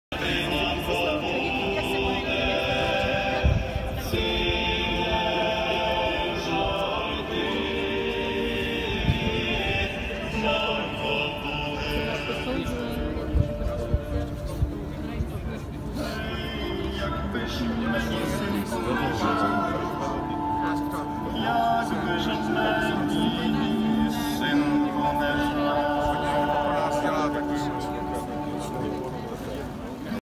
День памяти небесной сотни в Праге 2018 г.
День памяти небесной сотни на центральной площади в Праге 2018 г. Небесная сотня, это Украинцы, отдавшие свои жизни за независимость своей страны.